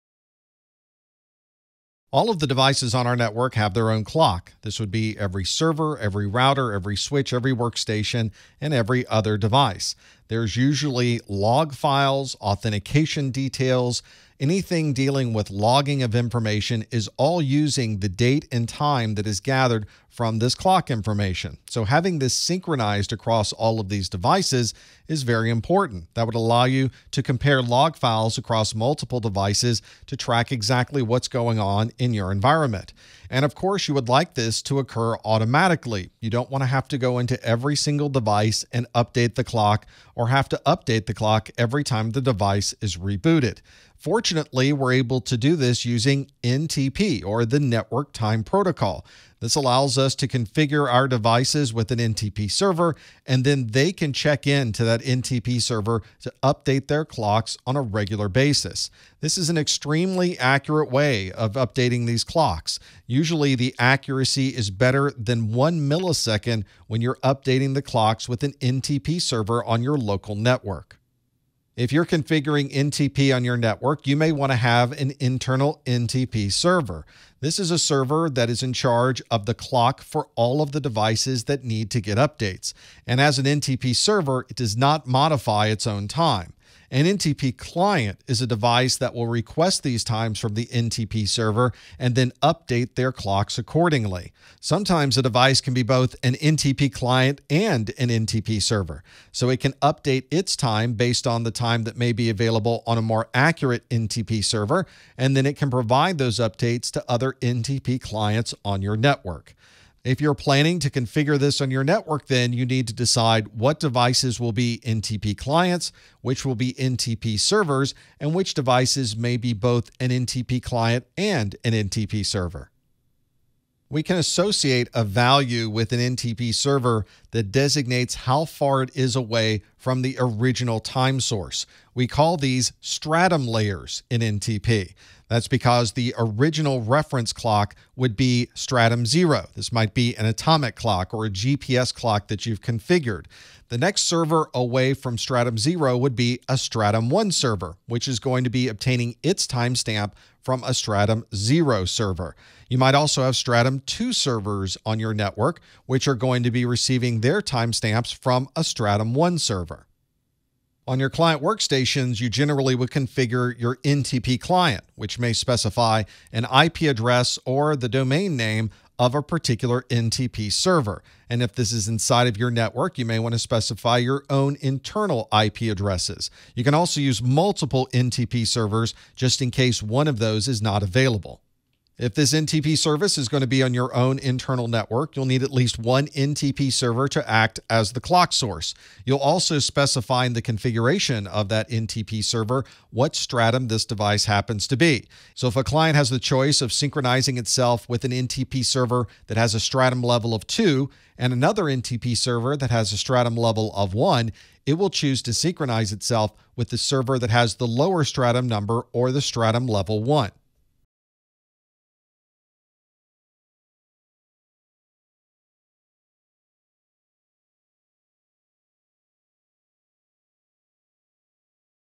An Overview of NTP - CompTIA Network+ N10-007 - 1.8
We rely on NTP to synchronize the clocks on all of our network-connected devices. In this video, you’ll learn about NTP servers, NTP clients, and how to configure NTP options on your workstation.